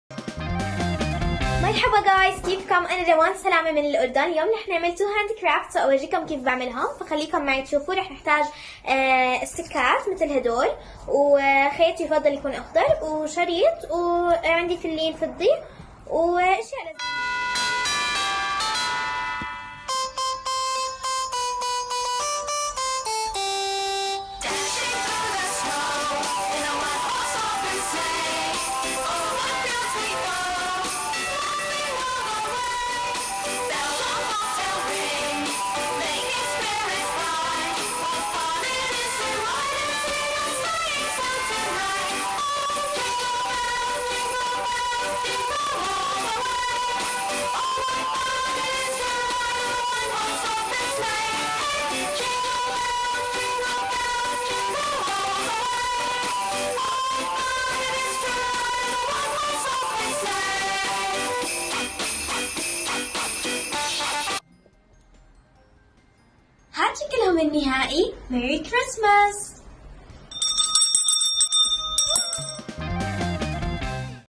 Bible Heroes | فيديو مشجّع من صديقتنا روان - أبطال الكتاب
👈فيديو من أصدقاءنا بيخبرونا كيف بيستفيدوا من وقتهم بالبيت بزمن الكورونا؟